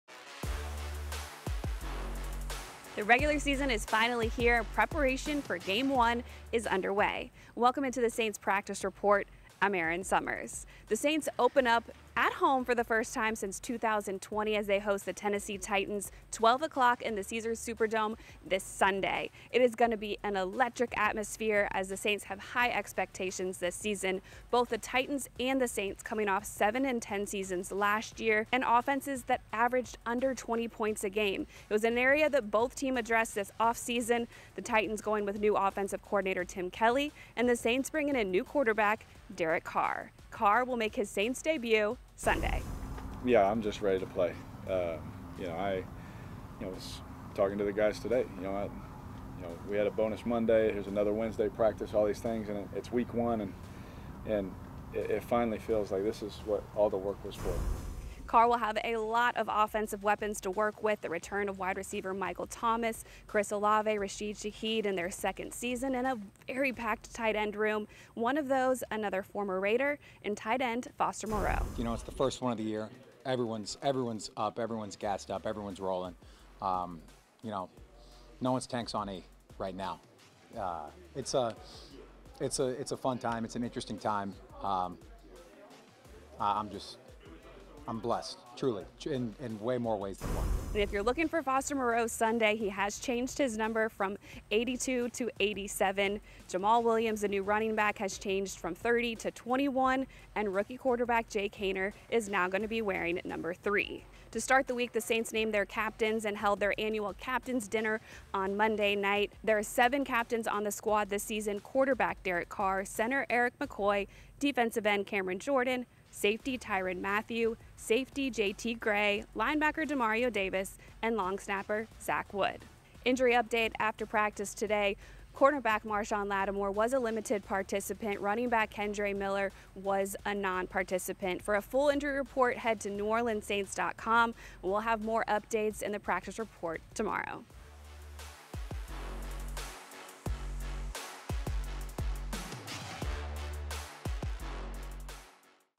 Saints Practice Report 9/6/2023 | Week 1 vs. Titans
New Orleans Saints Practice Report for Sep. 6, 2023 with Erin Summers ahead of the Saints' game vs. the Tennessee Titans in Week 1 of the 2023 NFL season.